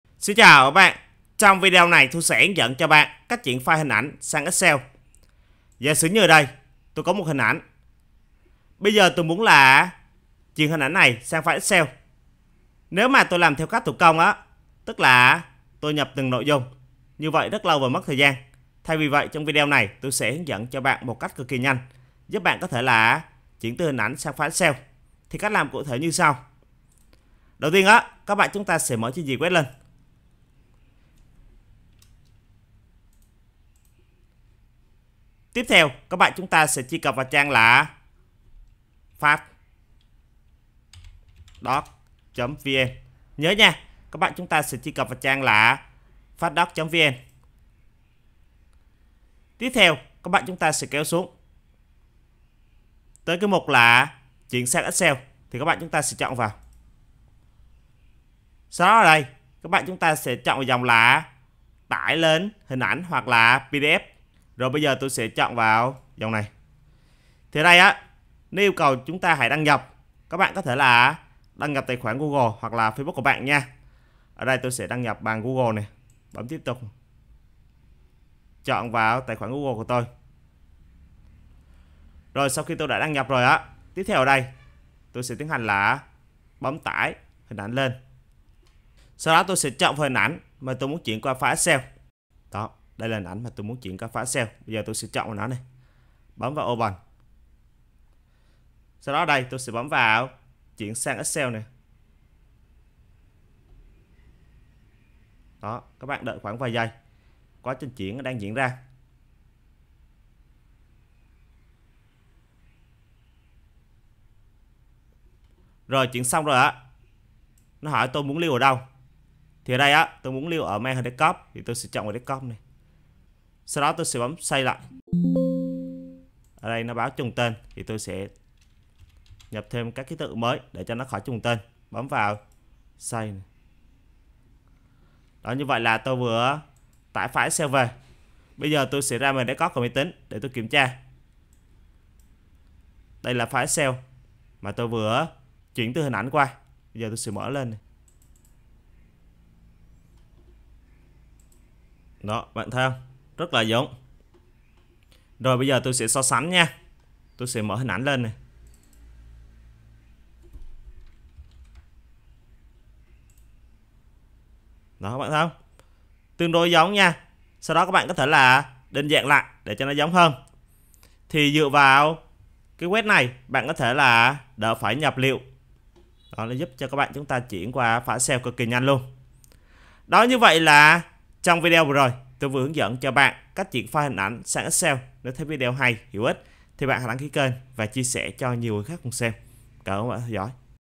Cách Chuyển File Ảnh Sang Excel Dễ Dàng
Trong video này mình sẽ hướng dẫn cho bạn, Cách Chuyển File Ảnh Sang Excel
Thực hiện bởi kênh kiến thức tin học